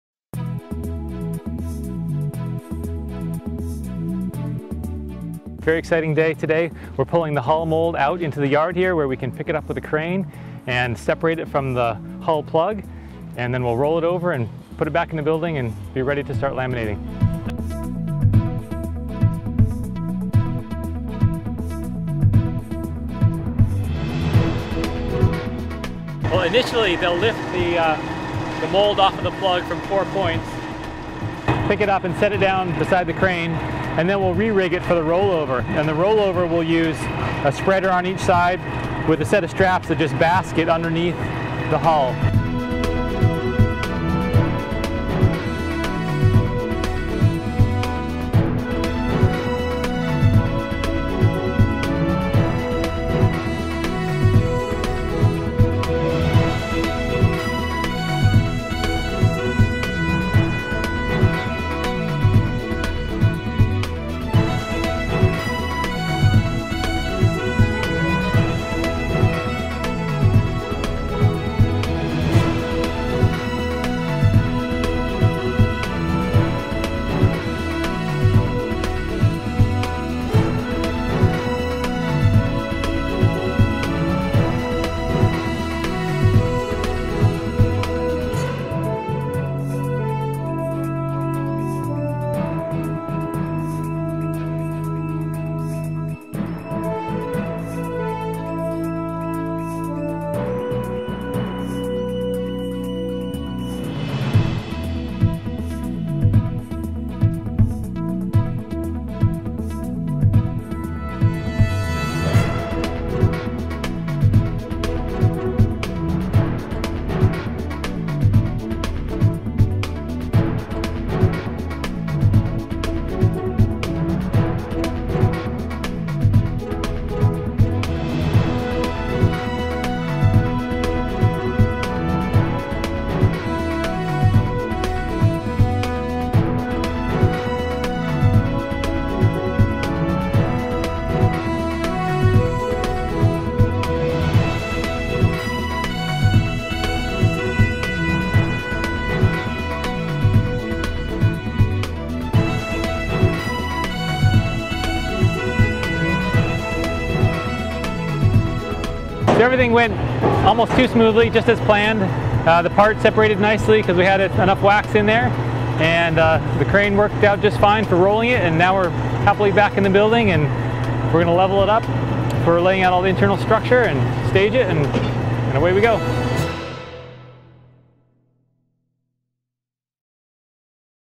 Kiwi Spirit Mold Pulling at Lyman-Morse Boatbuilding
Filmed at the Lyman-Morse Boatbuilding in Thomaston Maine on November 14 2011 This video by Billy Black shows us pulling the mold of the Paris 63' currently under construction at Lyman Morse.  Lance Buchanan, project manager, narrates.